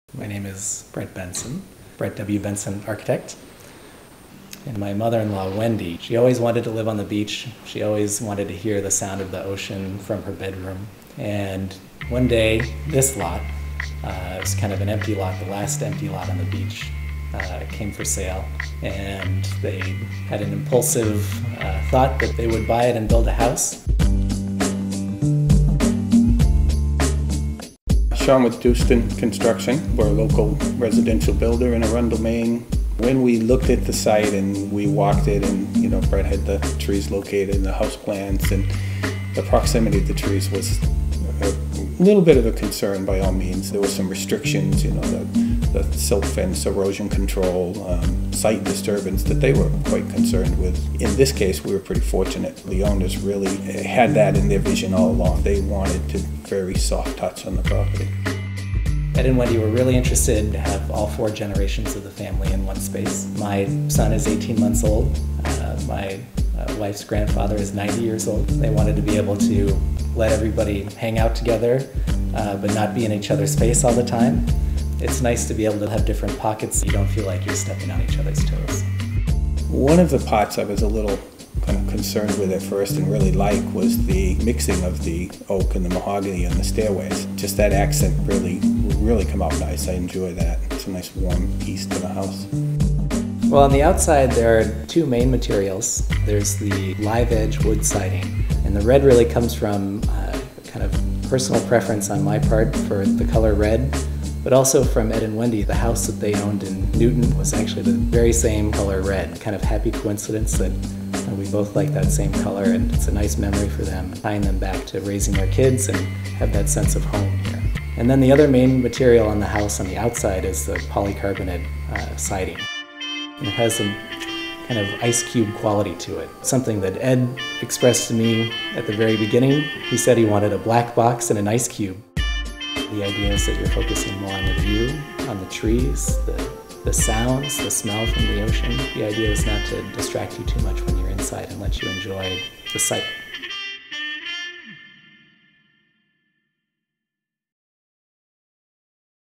Maine Home+Design's June 2012 Issue
A preview of the June issue of Maine Home+Design, featuring a Goose Rocks beach house designed by Brett Bentson and built by Douston Construction.